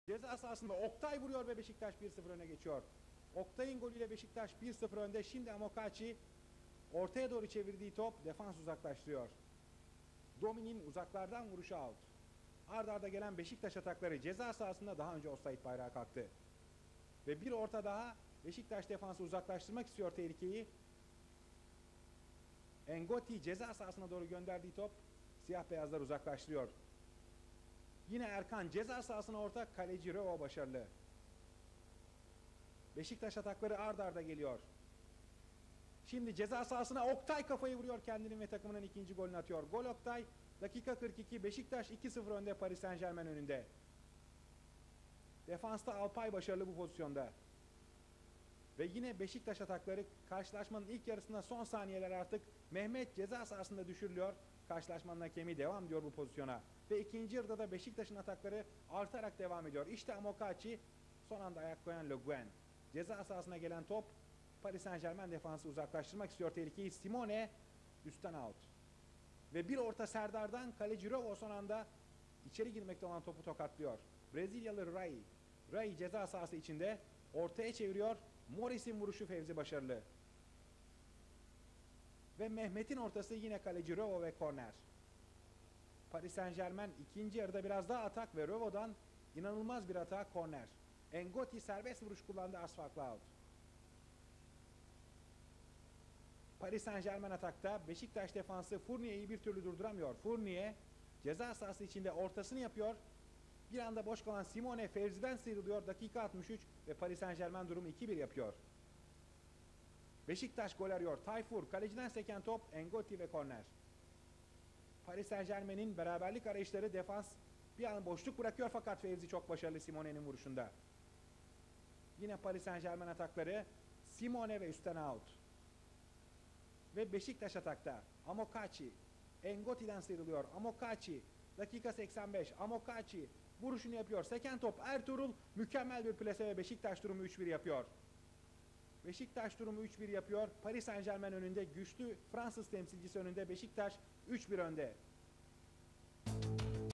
Beşiktaş 3-1 Paris Saint Germain (01.10.1997)
Beşiktaş'ın Şampiyonlar Ligi tarihindeki ilk galibiyeti.
Tarihinde ilk kez Lig formatında Kupa 1'de gruplara kalma başarısı gösteren Beşiktaş, İnönü'deki ilk maçında ünlü Fransız ekibi PSG karşısında net bir galibiyet elde ediyor..
Ancak grup sonunda Bayern Münih ve PSG 12'şer puanla ilk ikiyi, Bjk ile Göteborg 6'şar puanla son iki sırayı paylaşıyorlar..

Beşiktaş İlk 11:
Fevzi Tuncay
Recep Çetin
Zlatko Yankov
Alpay Özalan
Erkan Avseren
Rahim Zafer
Tayfur Havutçu
Mehmet Özdilek
Oktay Derelioğlu
Serdar Topraktepe
Daniel Amokachi
T.D.: John Benjamin Toshack

Paris SG İlk 11:
Christophe Revault
Paul Le Guen
Alain Roche
Bruno N'Gotty
Didier Domi
Laurent Fournier
Rai
Franck Gava
Edmilson
Marco Simone
Florian Maurice
T.D.: Ricardo Gomes

Maçın Hakemi: Günte Benko
Goller: Oktay Derelioğlu(6' ve 42'), Ertuğrul Sağlam(84'); Marco Simone(65')